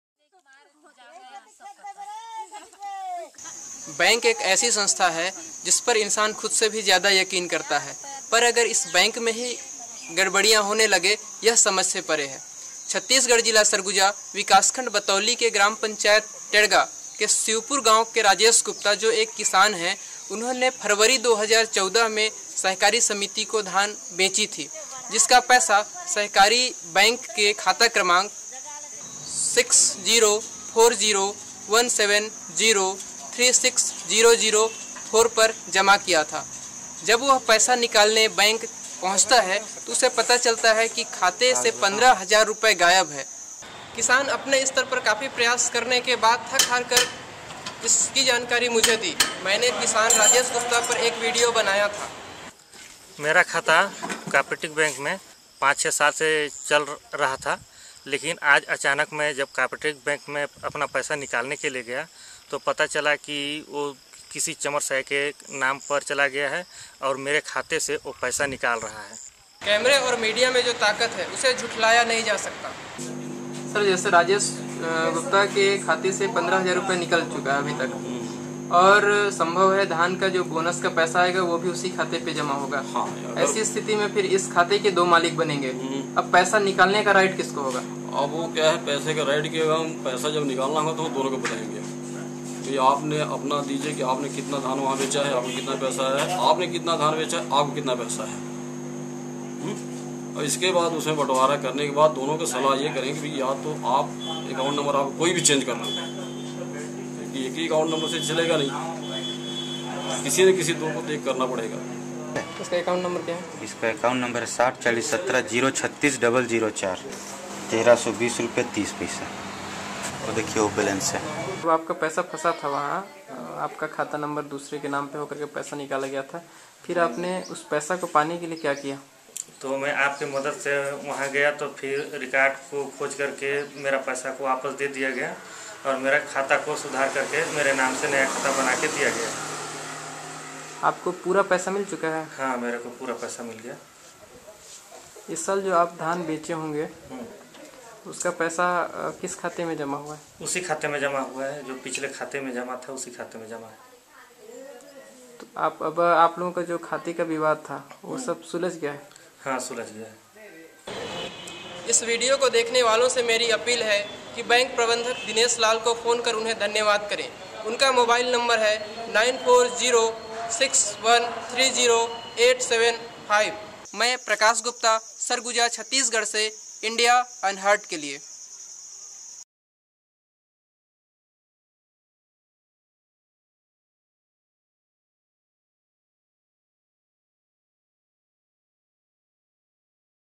Impact_Money Refunded by Bank - Prakash Gupta Reports for IndiaUnheard
Rajesh Gupta a farmer received his all money from bank which was withdraw by another person from Shivpur village of Sarguja district of Chhattisgarh.

Community Correspondent Prakash Gupta reports from Chhattisgarh for IndiaUnheard.